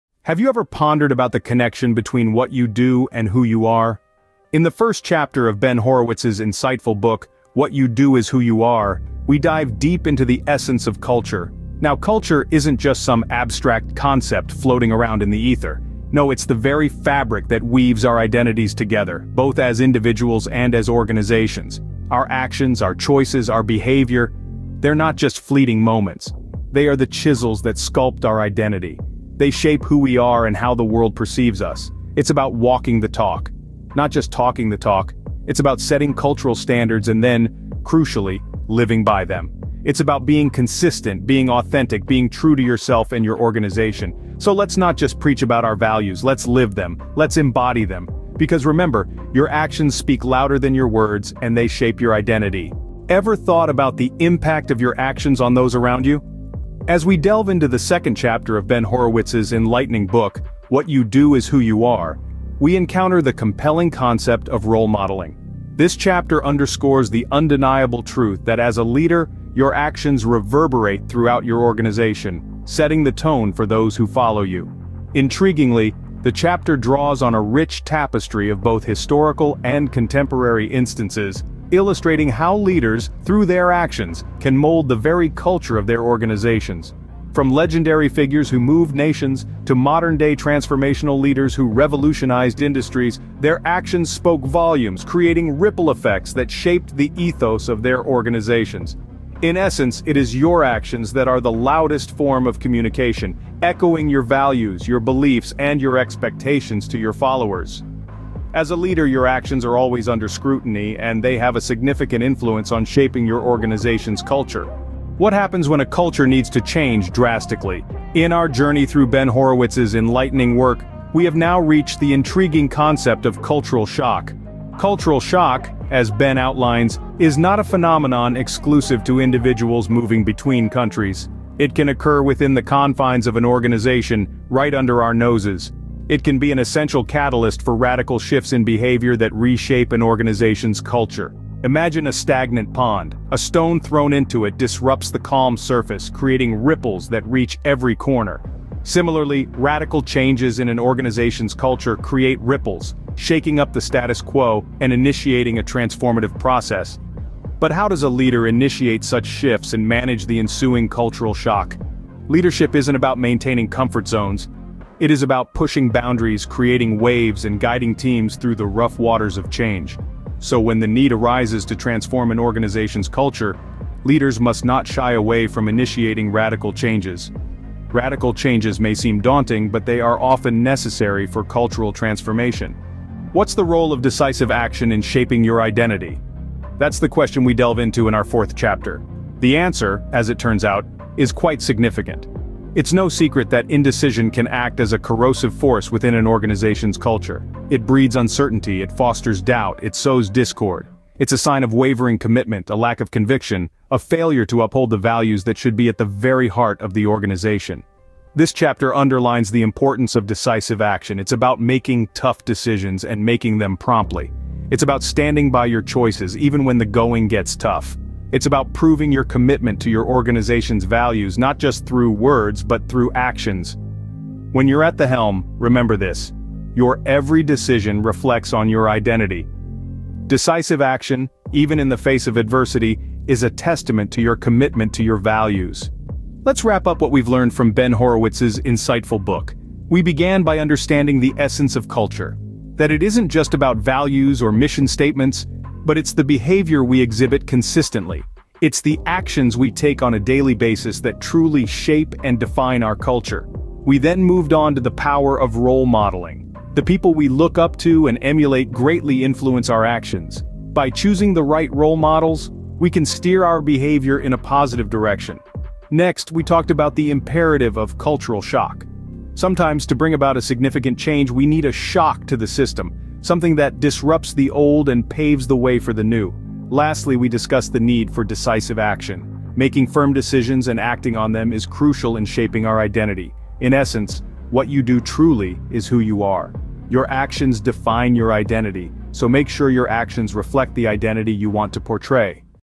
What You Do Is Who You Are by Ben Horowitz | Book Summary
In "What You Do Is Who You Are," Ben Horowitz delves into the critical importance of company culture and leadership in shaping organizational identity. Drawing from historical examples and his own experiences as a tech entrepreneur and investor, Horowitz explores how leaders can create and sustain a positive culture that aligns with their values and goals. Through engaging anecdotes and practical insights, he reveals the key principles and strategies for building a strong culture, fostering accountability, and driving organizational success. "What You Do Is Who You Are" is a compelling guide for leaders seeking to cultivate a culture that reflects their ethos and enables their teams to thrive in today's competitive business landscape.